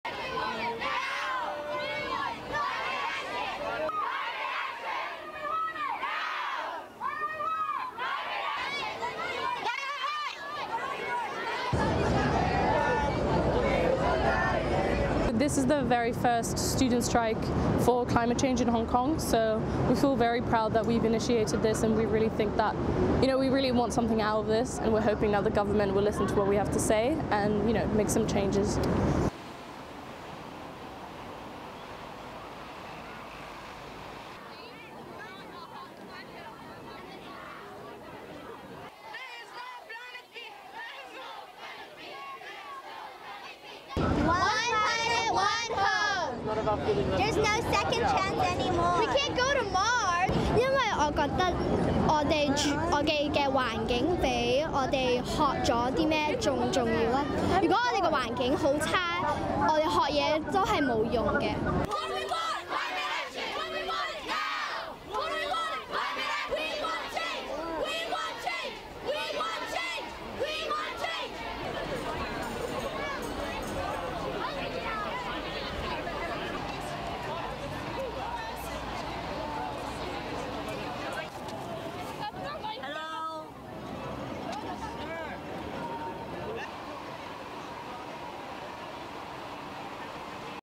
HKong students call for action on climate change
(15 Mar 2019) About 200 students gathered in Hong Kong on Friday, calling for better government action against climate change.
Inspired by 16-year-old Swedish climate activist Greta Thunberg, students participated in the school strike urging the Hong Kong government for immediate actions to prevent climate change.
The strike, also known as Schools Strike For Climate, is part of the global movement FridaysForFuture.
Students boycotting schools gathered at 0300GMT at Chater Garden in central Hong Kong. After an hour of gathering, the crowd started marching towards Hong Kong Central Government Office to show their concerns over the lack of action in Hong Kong against climate change.